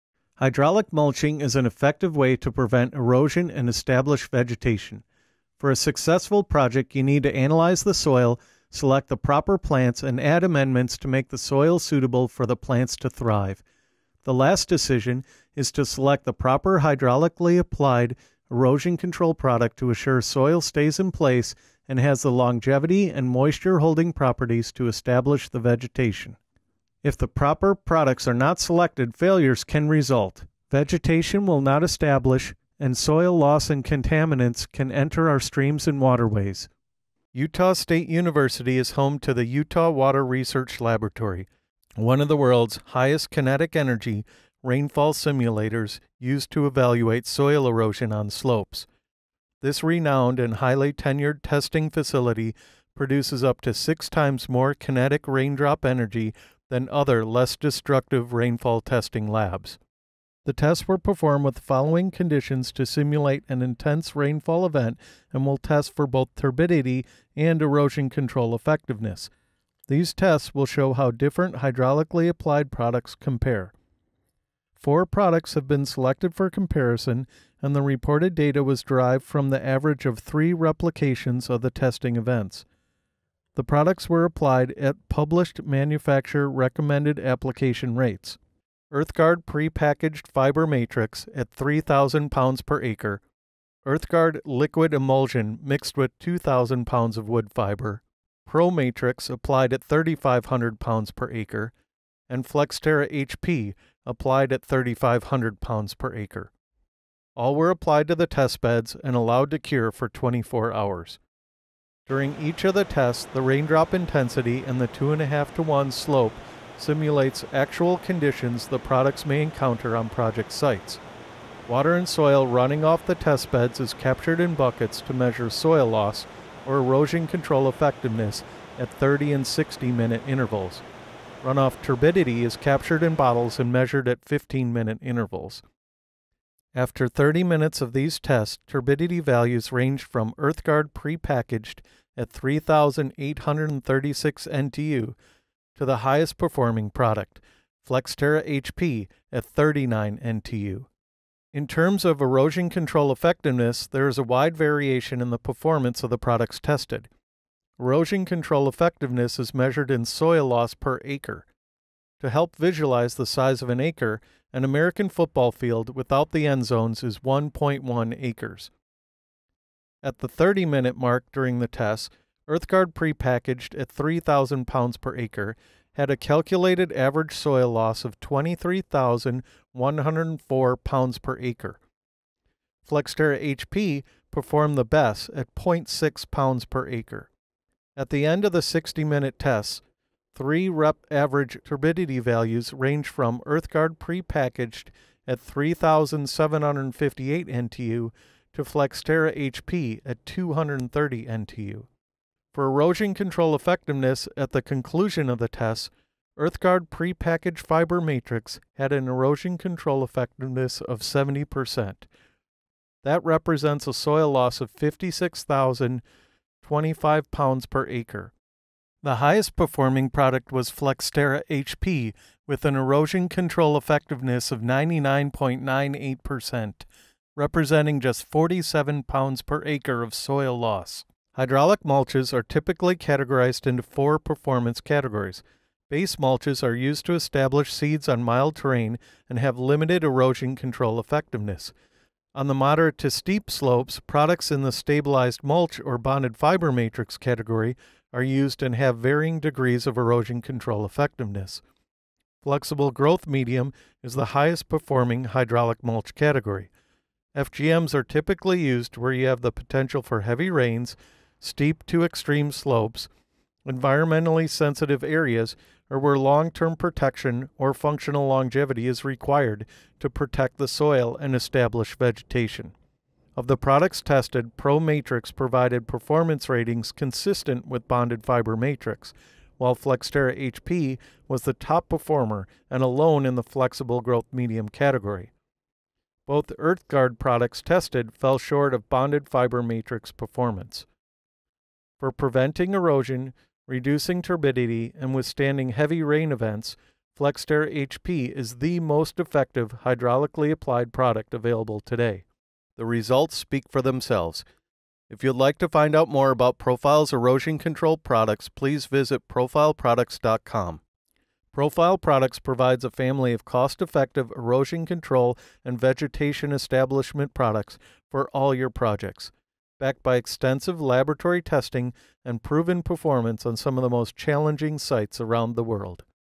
Side-by-Side Comparison of Erosion Control Product Effectiveness
Hydraulically applied mulch is an effective way to prevent erosion and provide sustainable vegetation. When selecting the right erosion control product for your site, choosing a product with the proper erosion control effectiveness is paramount to your success. In this side-by-side comparison, EarthGuard® Fiber Matrix Pre-Packaged*, EarthGuard® Fiber Matrix Liquid, ProMatrix™ Engineered Fiber Matrix™† and Flexterra® High Performance-Flexible Growth Medium®‡ were tested at the independent Utah Water Research Laboratory at Utah State University, one of the world’s highest kinetic energy rainfall simulators used to evaluate soil erosion on slopes.

Each product was applied at the published manufacturer rates and tested on a 2.5H:1V slope with sandy loam soil. The lab simulated 5 inches (127 mm) of rainfall per hour for 60 minutes.

The products performed as follows, from most to least soil loss:
• EarthGuard® Fiber Matrix Pre-Packaged had an erosion control effectiveness of 70%, representing a soil loss of 56,025 lb/ac (62,748 kg/ha). This product performed at the level of a Stabilized Mulch Matrix.
• EarthGuard® Fiber Matrix Liquid had an erosion control effectiveness of 86%, representing a soil loss of 26,893 lb/ac (30,120 kg/ha). This product performed at the level of a Stabilized Mulch Matrix.
• ProMatrix™ EFM™ had an erosion control effectiveness of 96%, representing a soil loss of 7,428 lb/ac (8,320 kg/ha). This product performed at the level of a Bonded Fiber Matrix — one level higher than a Stabilized Mulch Matrix.
• Flexterra® HP-FGM® had an erosion control effectiveness of 99.98%, representing a soil loss of 47 lb/ac (53 kg/ha). This was the only product to perform at the level of a Flexible Growth Medium, the highest-performing hydraulic mulch category.

The results speak for themselves. For preventing erosion, reducing turbidity and withstanding heavy rain events, Profile’s erosion control products are proven to hold soil in place and help you succeed.

* EarthGuard is a registered trademark of LSC Environmental Products LLC.
† ProMatrix, Engineered Fiber Matrix and EFM are trademarks of PROFILE Products LLC.
‡ Flexterra, High Performance-Flexible Growth Medium and HP-FGM are registered trademarks of PROFILE Products LLC.